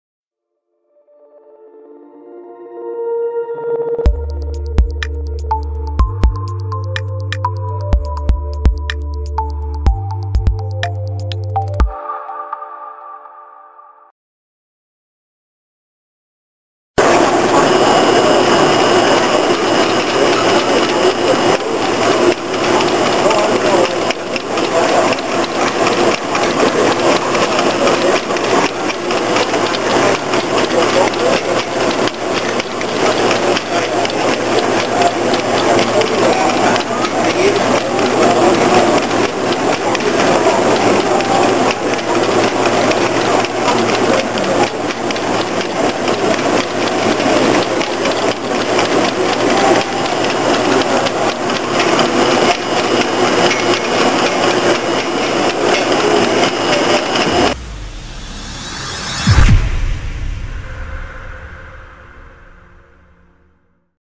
Automated Scratch Off Machine from Pineberry
This is a personalization system for cards from Pineberry Manufacturing. Excellent for adding a pin number and scratch off labels or foil to phone cards, gift cards and more. The system can also use high end DOD inkjet for full personalization of cards.